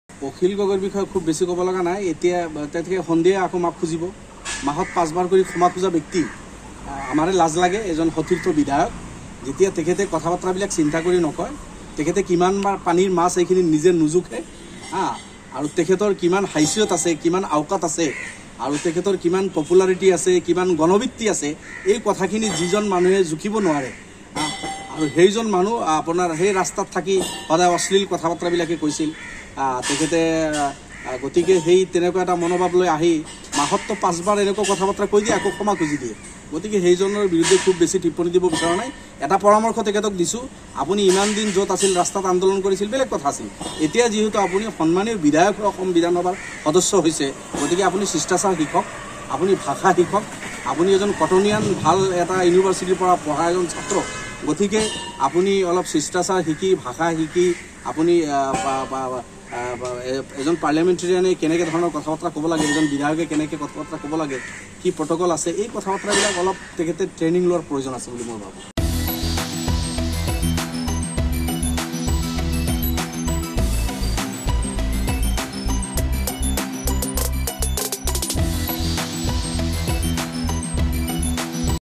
এমাহত ৫ বাৰকৈ ক্ষমা খোজা মানুহ অখিল গগৈ : ৰফিকুল ইছলাম
▶️এমাহত ৫ বাৰকৈ ক্ষমা খোজা মানুহ অখিল গগৈ। কংগ্ৰেছৰ নেতৃত্বৰ ভুলৰ বাবেই বিজেপিৰ উত্থান হৈছে। কংগ্ৰেছে অহংকাৰ কৰি লাভ নাই, বৰ্তমান কংগ্ৰেছৰ শক্তি আৰু AIUDFৰ শক্তি সমান বুলি দাবী ৰফিকুল ইছলামৰ।